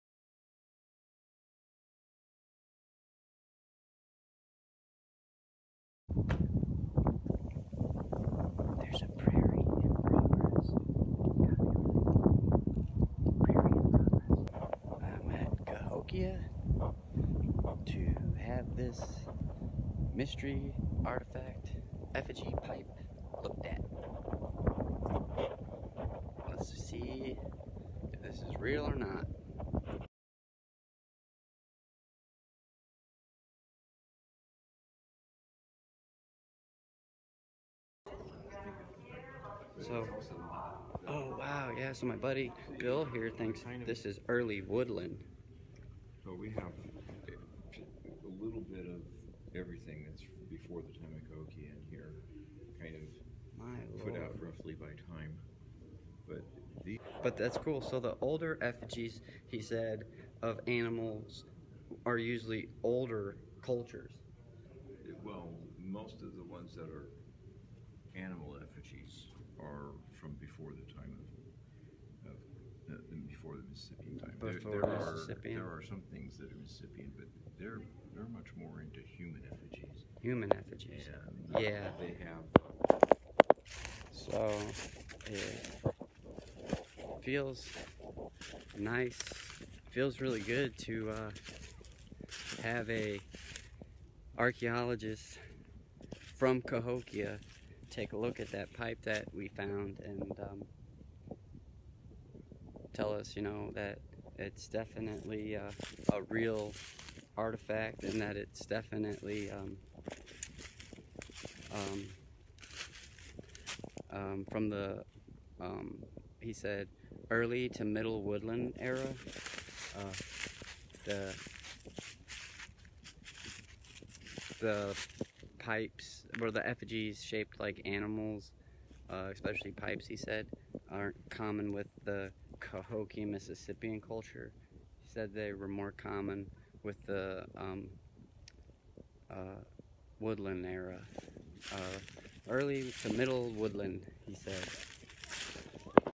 Effigy pipe
Took the mystery pipe to Cahokia to have archeologists examine the pipe. They said it’s not Mississippian or Cahokian it’s older and it’s from the early to middle woodland era and that it’s a real authentic artifact several other archeologists from Wash-U said the same thing it’s now in a safe place.